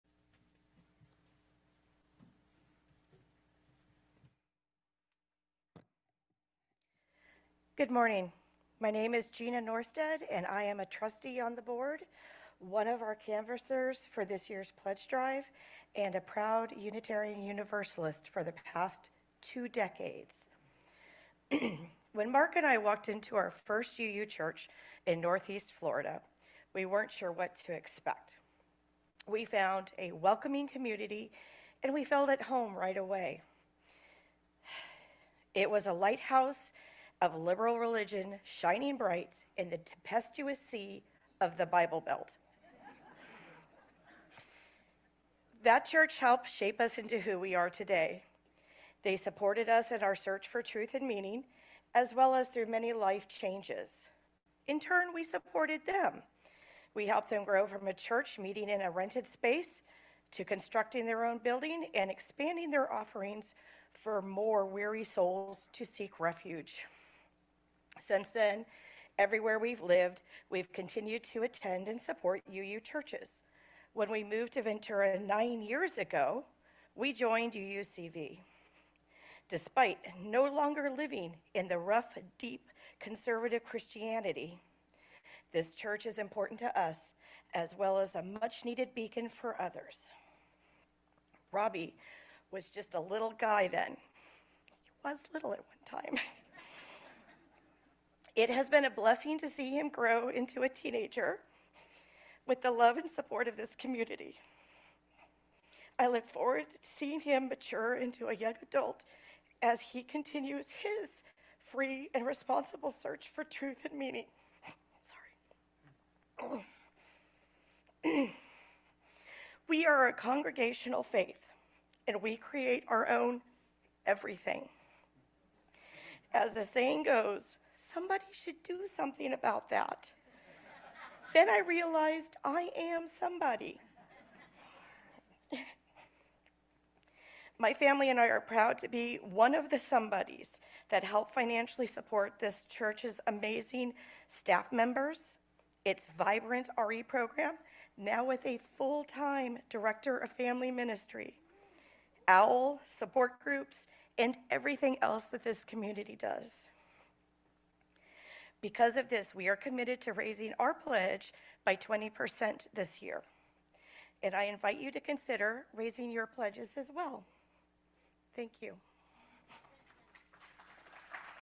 Gina Norstedt - Mar. 24, 2024
Gina Norstedt on her commitment to UUCV.